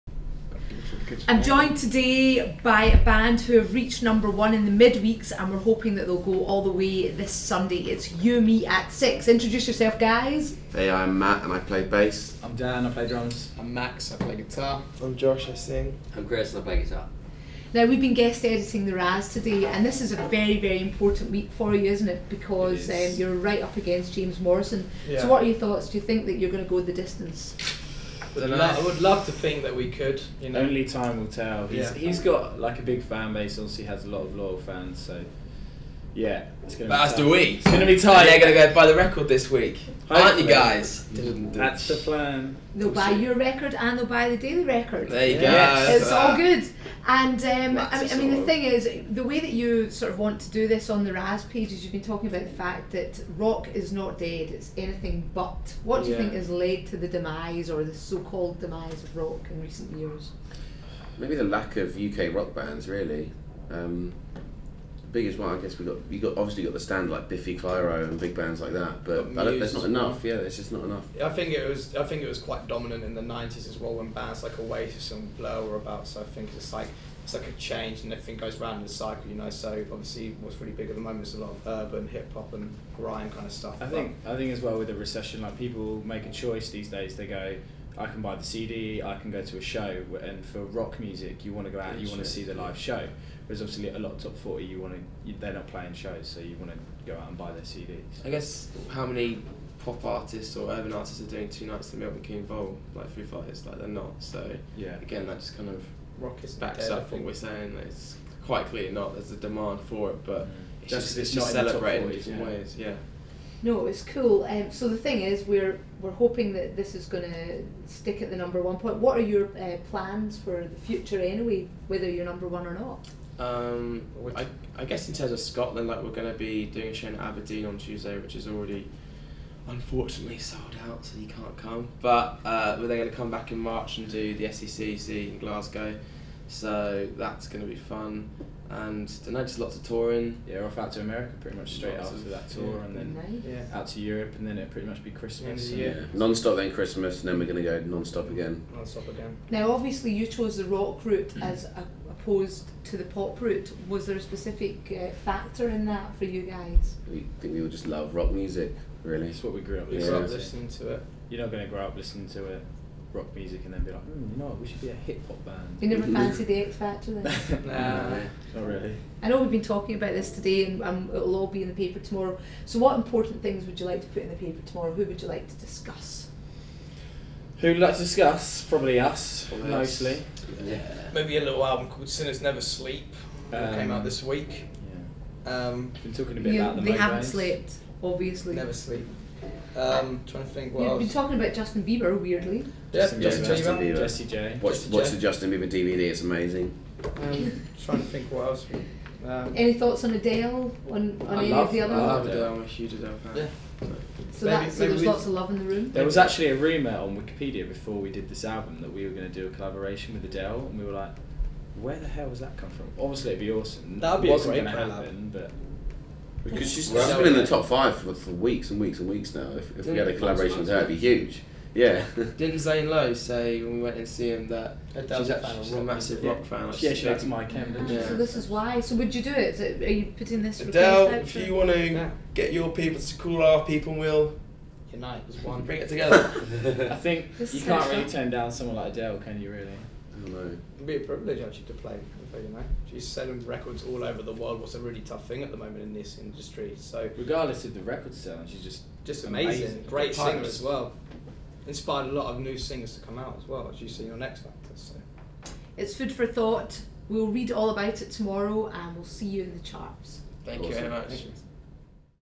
You Me At Six guest edit The Razz
Rockers You Me At Six take a break from their whistle-stop promo tour of Scotland to help choose the top showbiz stories for the Daily record's entertainment guide, The Razz.